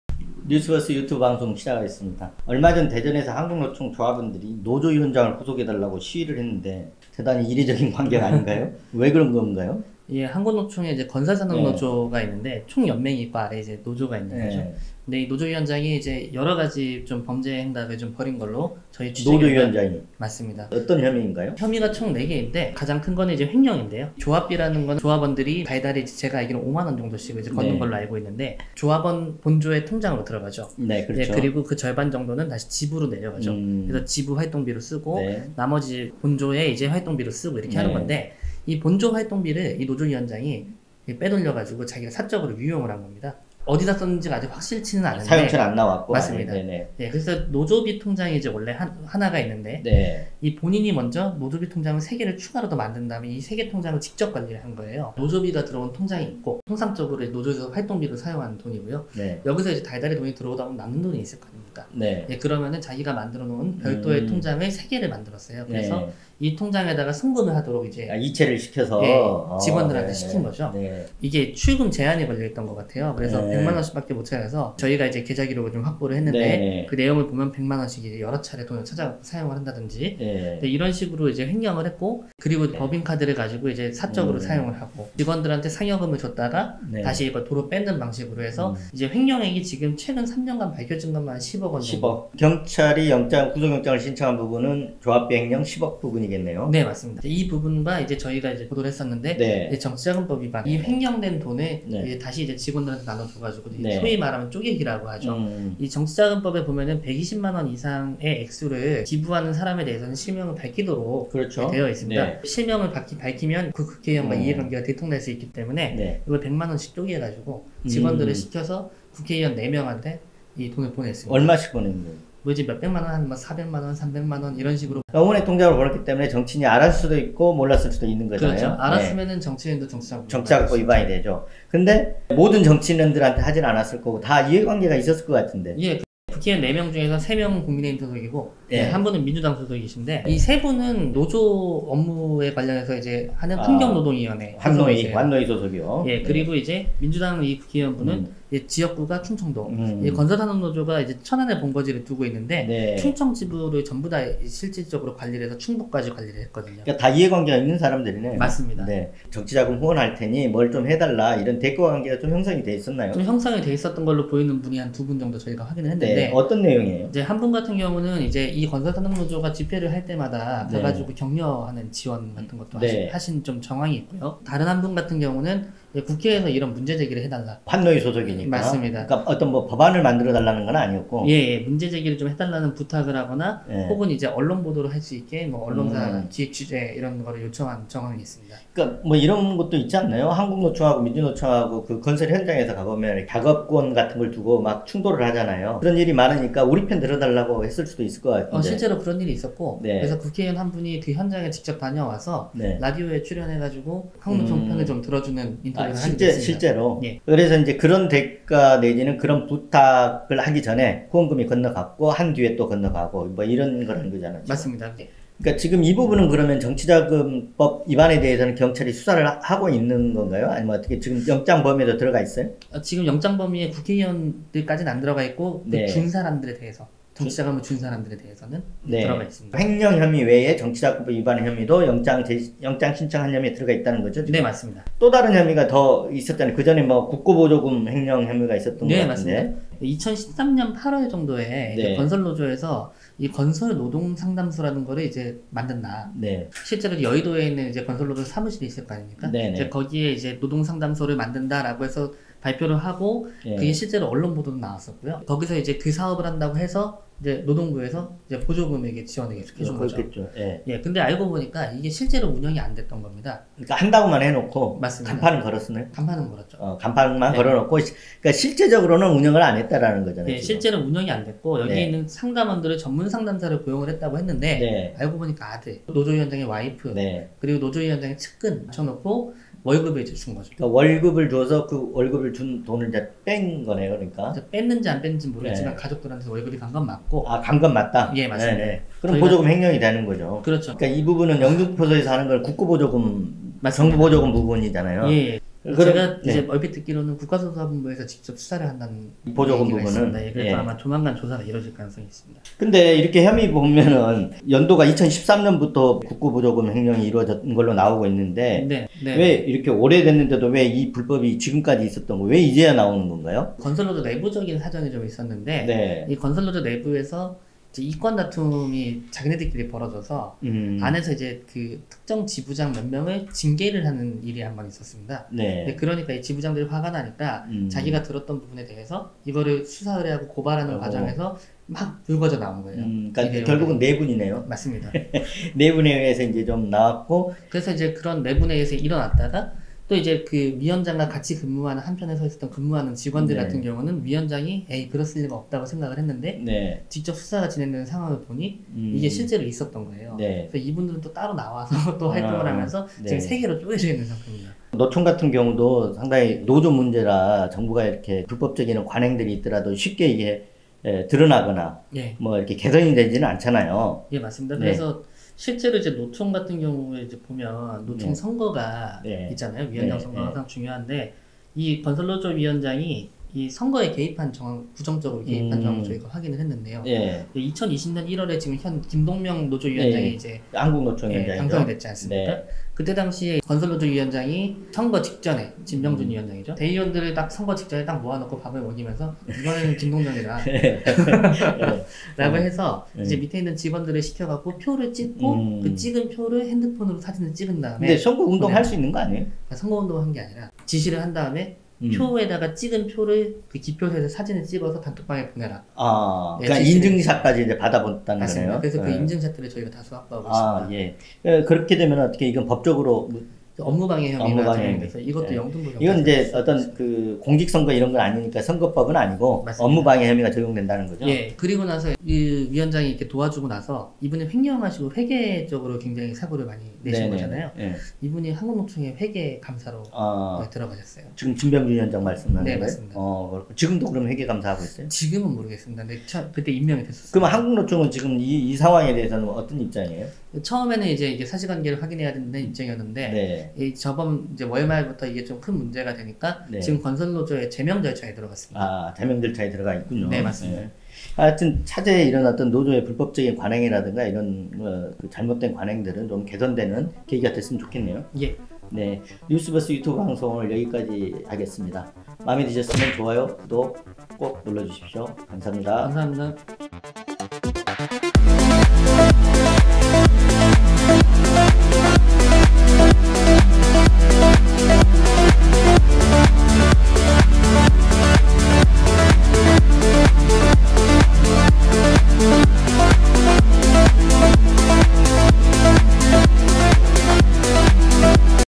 한국노총건설위원장 구속되나... 어쩌다? 조합비 야금야금 10억 횡령
'10억대 횡령' '정치인 불법 후원' 혐의
'건설노조, 환노위 소속  의원에게도 '쪼개기' 수법 정치자금 제공
타워분과, 후원 뒤 "민주노총 불법행위 공론화해달라"민원